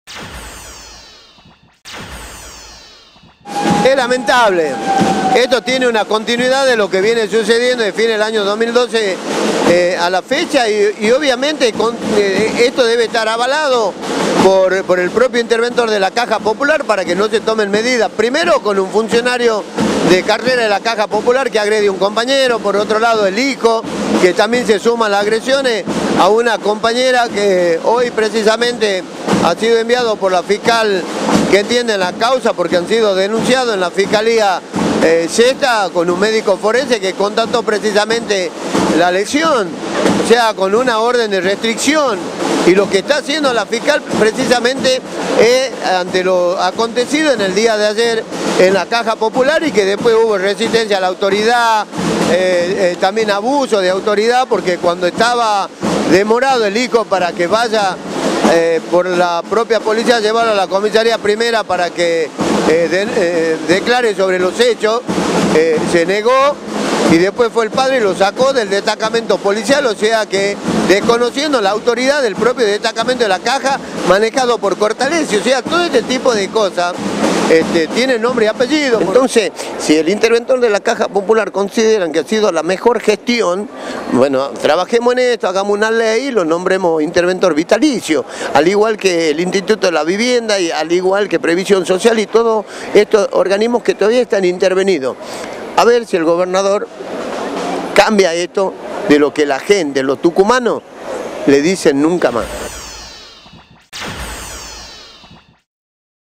Volvieron las protestas de la Bancaria en Caja Popular
Una protesta de La Bancaria complicó esta mañana el tránsito en San Martín y 25 de Mayo, en la puerta de la Caja Popular de Ahorros. Denuncian maltrato y agresiones a empleados bancarios por parte de funcionarios de la institución.